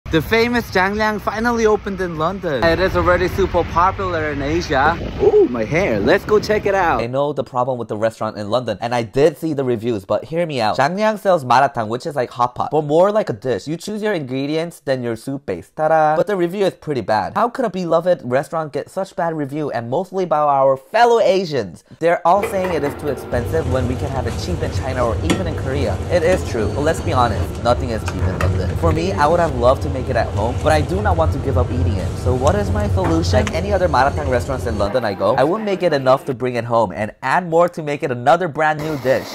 Zhang Liang Malatang In London Got Bad Reviews, WHY?
zhangliang is one of the biggest malatang chain in Asia. I used to go in Korea and I was excited when they opened here.

I know it is more expensive than Asia, but what isn’t expensive in London? 🤪 I know you can make it at home, but I wouldn’t have much time to prepare all the ingredients.

So, when I go to Malatang restaurants in London, I normally get about two portion size and take it to go, so I can add more ingredients to make it another dish.